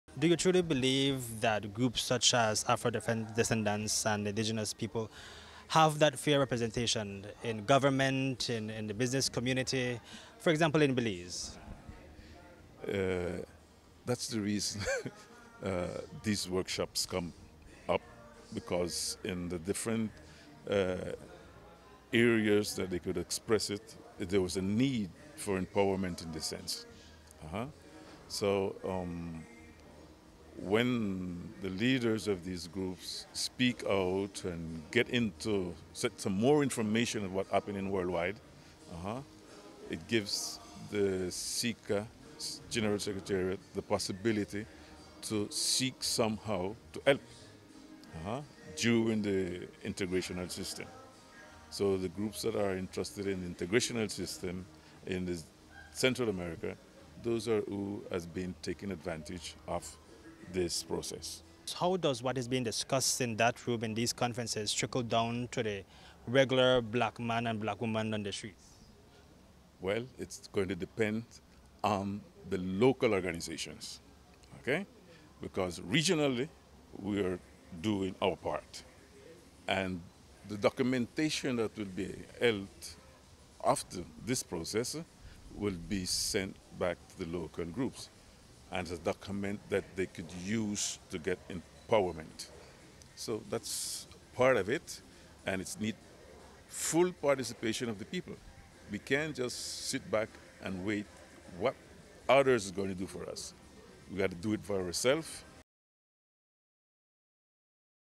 International Decade for People of African Descent
The UN General Assembly proclaimed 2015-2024 as the International Decade for People of African Descent citing the need to the full enjoyment of rights such as economic, social and political and their full and equal participation in all aspects of society. In the region, the Central American Black Organization, CABO, through the larger body SICA, is hosting conferences with local groups to promote respect, protection, and fulfillment of all human rights and fundamental freedoms by people of African Descent. Today is the final day of the two-day conference and News Five spoke to CABO representative, Edly Hall.